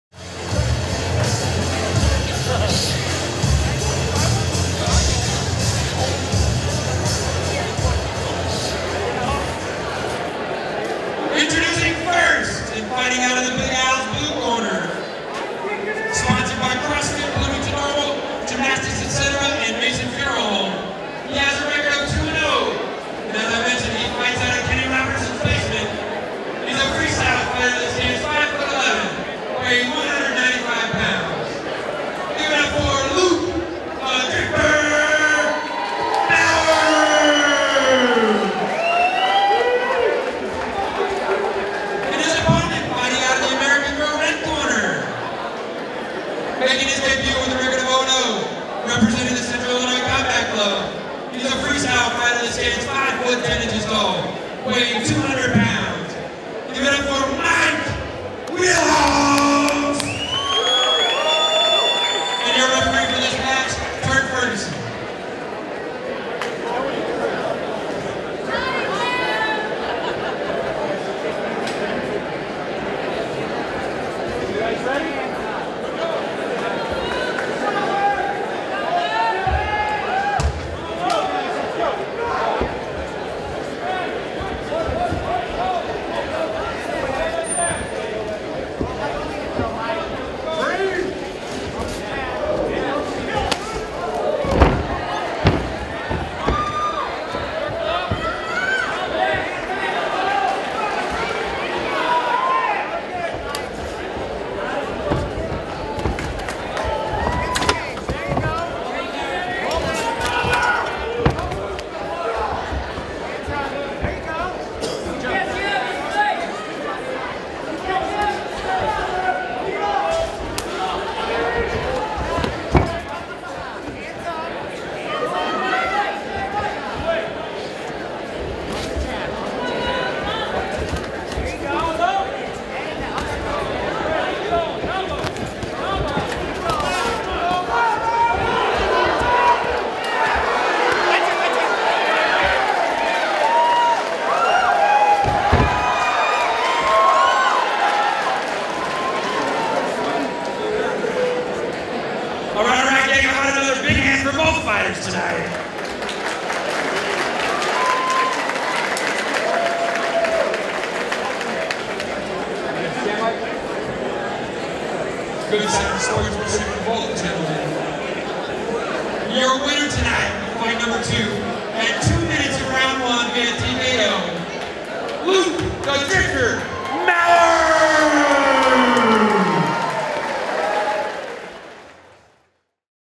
Central Illinois Throwdown XVII - Fight 2: Richard Wheelhouse vs Luke Mauer
United Combat League Presents:
Central Illinois Throwdown XVII
Saturday, October 18th 2014
Peoria Expo Gardens